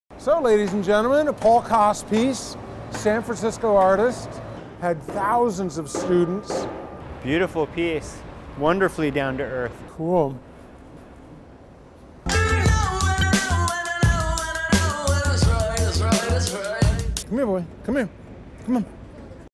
Paul Kos / March 30th Launch
Featuring Paul Kos' BIG BERTHA at 199 Fremont Street with artists, Chris Johanson and Kal Spelletich.  *Video by Packard Productions